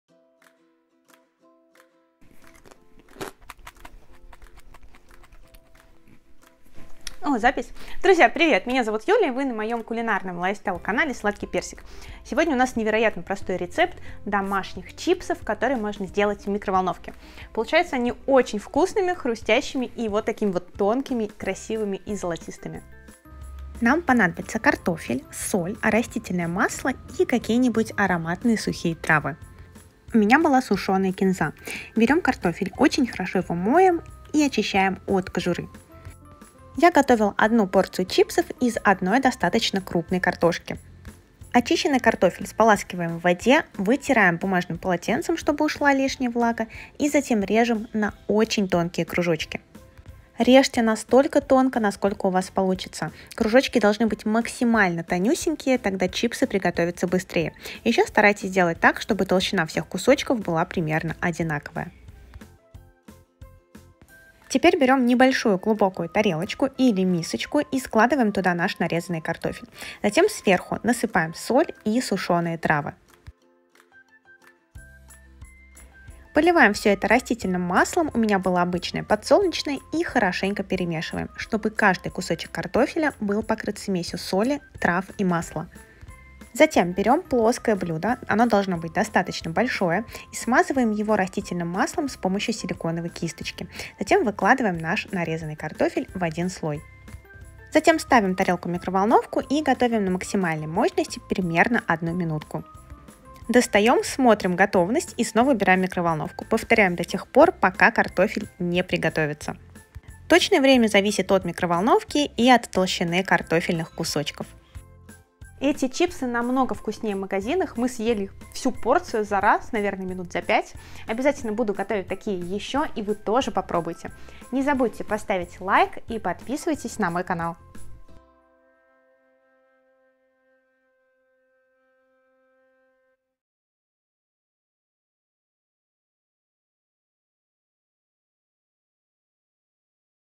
Настоящие ДОМАШНИЕ ЧИПСЫ в микроволновке / chips
Рецепт чипсов, которые можно приготовить в домашних условиях. Эти картофельные чипсы получаются вкуснее и полезнее магазинных! Приготовьте, будем готовить чипсы в микровлоновке =)
Что нужно:
картофель – 1 крупная штука
соль – 1/2 ч.л. или по вкусу
сушеная кинза (или любые другие сухие травы) – 1/2 ч.л.